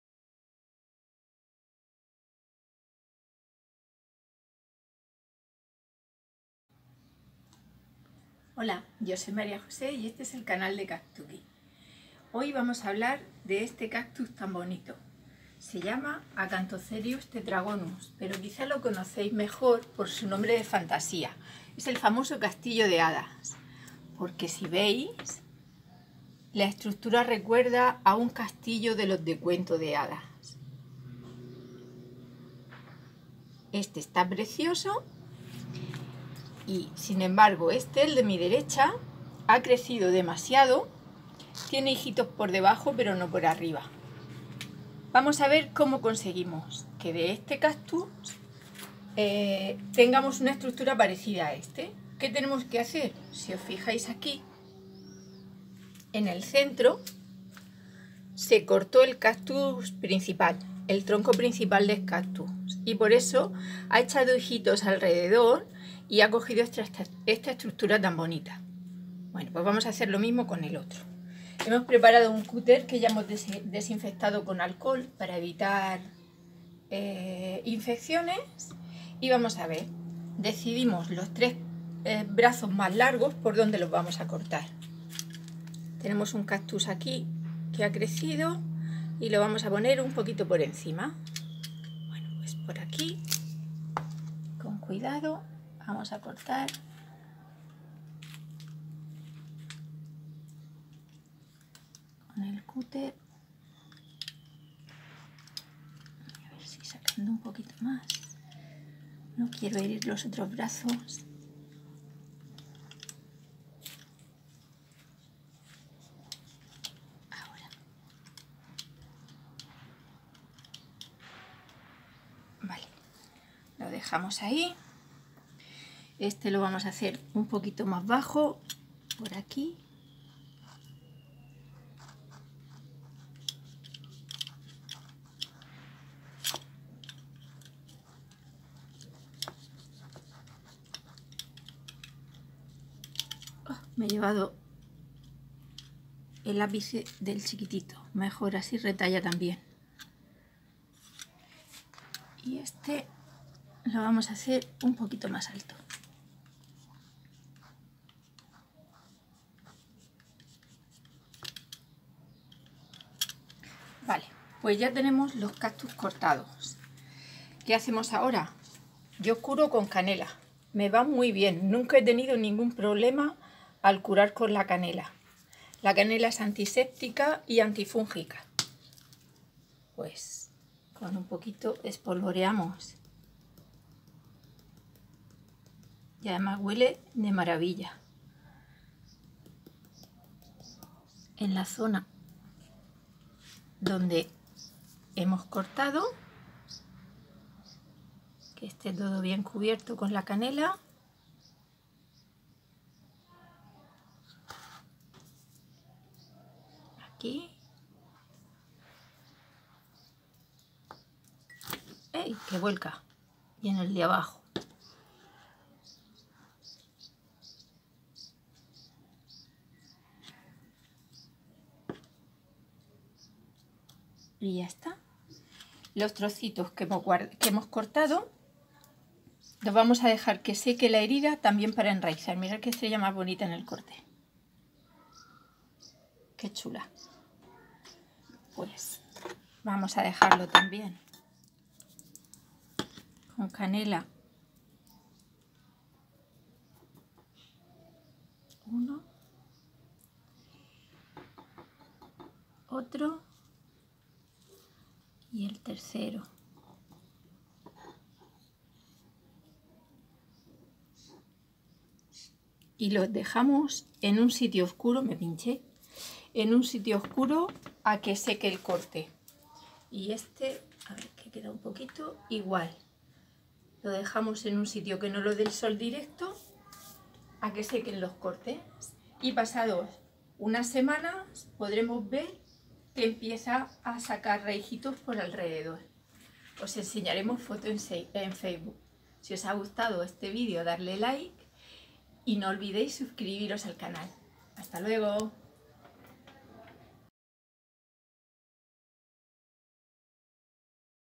Poda del Castillo de Hadas. Acanthocereus tetragonus. | Caktuky
Hoy mostramos un tutorial sobre la manera de podar el Castillo de Hadas para que conserve su bonita estructura.